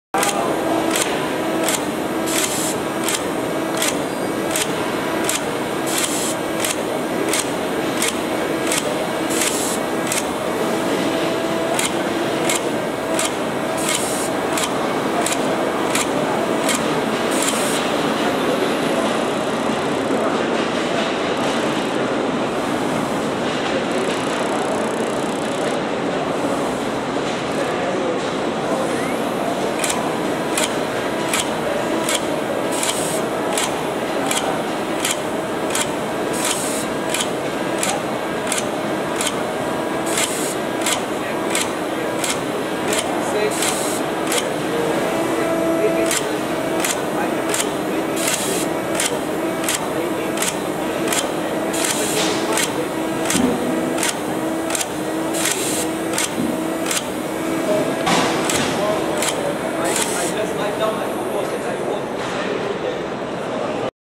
TWISTER fully automated gluing and rubbering system
The Worlds first and only fully automatic gluing and rubbering system!!
This system is the final piece in the jigsaw offering die makers the opportunity to fully automate the whole die making process.
Gluing and rubbering on large multi up jobs can take hours to complete, with the Twister this can be done in minutes!!
The machine uses the same drawing (DXF, CF2 etc) that is used with your EasyBender and Laser cutting system.